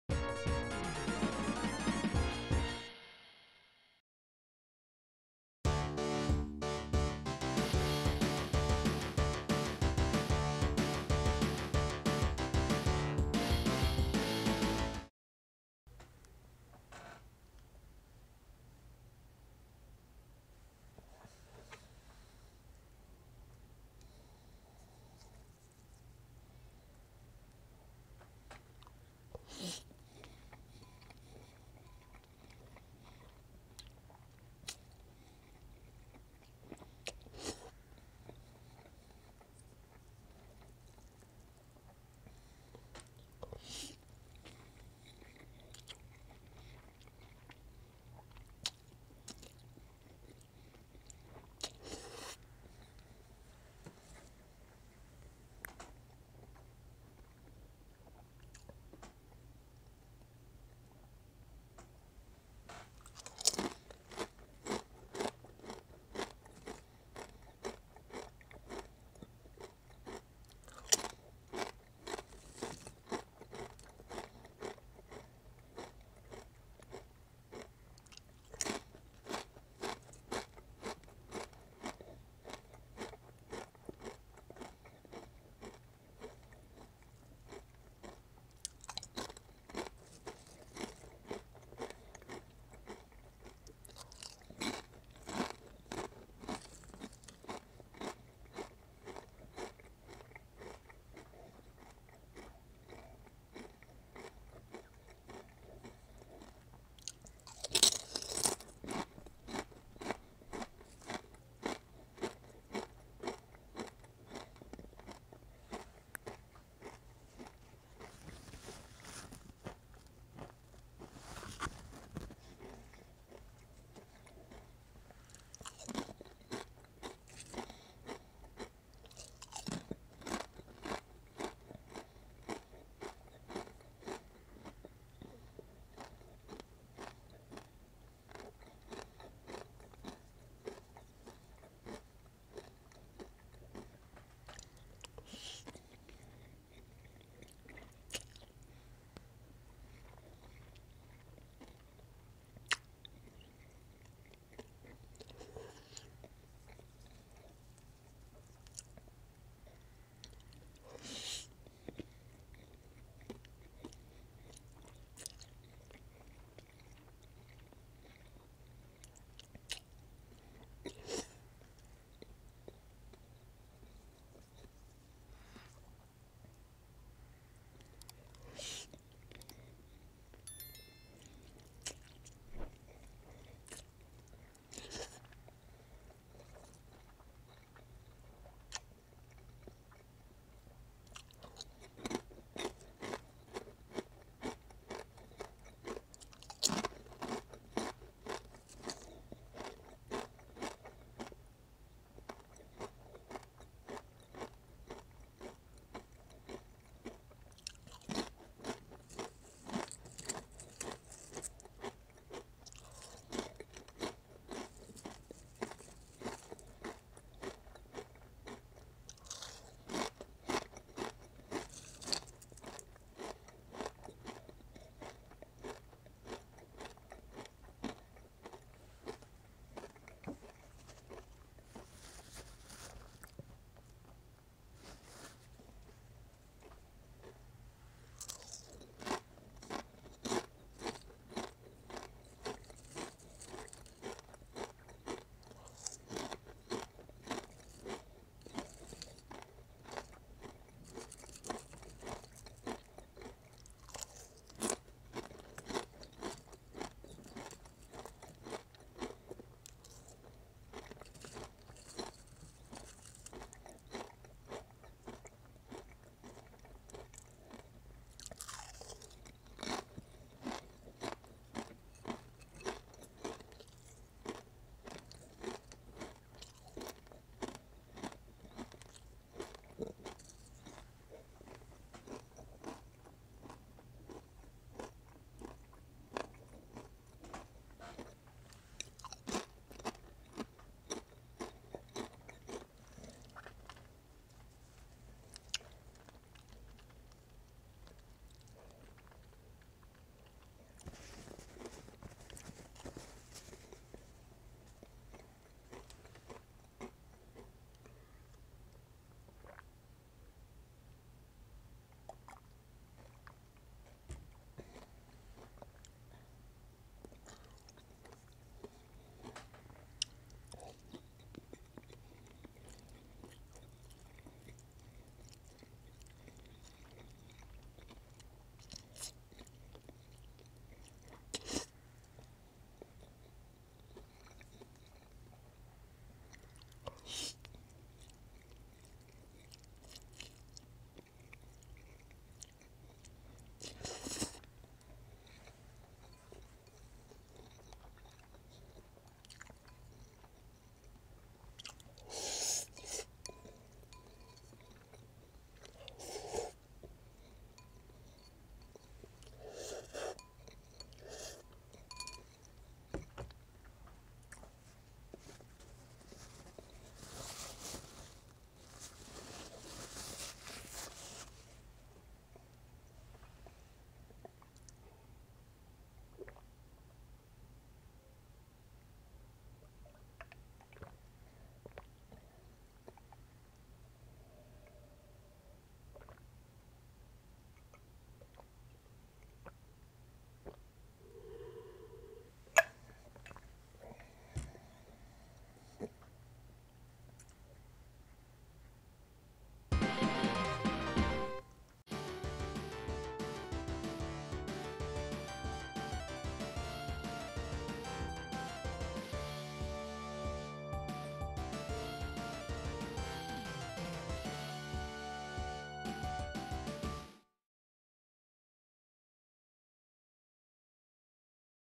LUCKY ME! PANCIT CANTON SWEET & SPICY & MISS VICKIE'S JALAPENO ASMR MUKBANG
Today we have a couple pack of Lucky Me! Pancit Canton Sweet & Spicy & some Miss Vickie's Jalapeño with Chocolate Milk to drink!